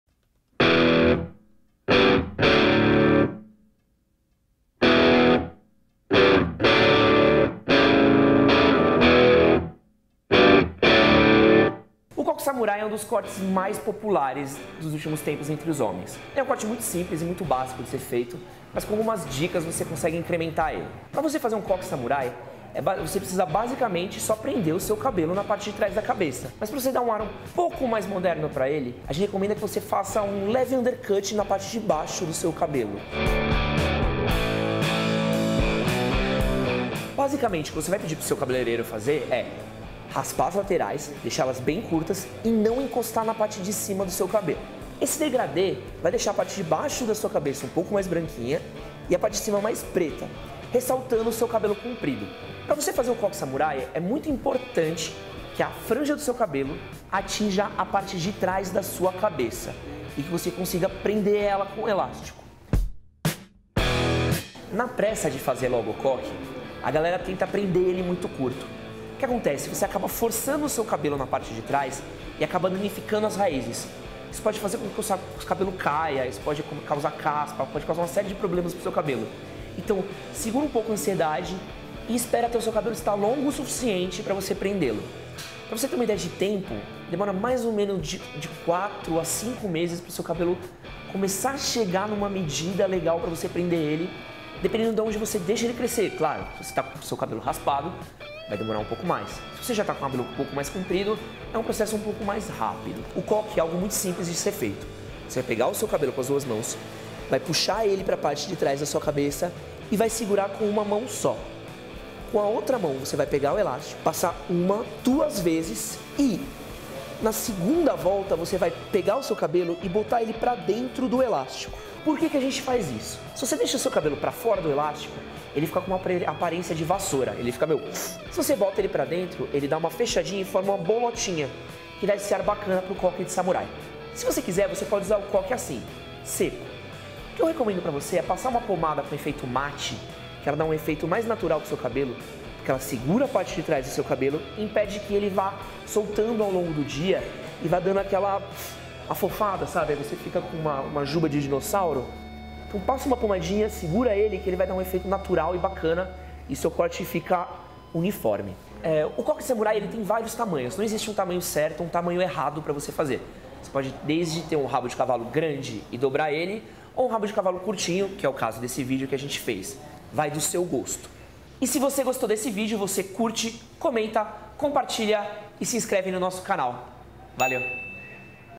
Como fazer o Coque Samurai
O coque samurai é uma das maiores tendências de corte de cabelo masculino nos últimos tempo. O Eddie foi até a Barbearia Sport Club para mostrar com você deve fazer para ter um belo coque masculino em sua cabeça..

Como fazer o coque masculino? Como transformar o coque samurai em um corte masculino moderno? Confira nosso vídeo e veja como adotar o coque samurai, um dos cortes masculinos que mais prometem se destacar em 2016.

► BARBEARIA SPORT CLUB
► ENDEREÇO: Rua Itaqueri, 1328 - Mooca - , São Paulo. SP

► Deixa ai o seu CURTIR, se você gostou do vídeo, é claro.
► Deixe o seu COMENTÁRIO e junta-se a nossa conversa.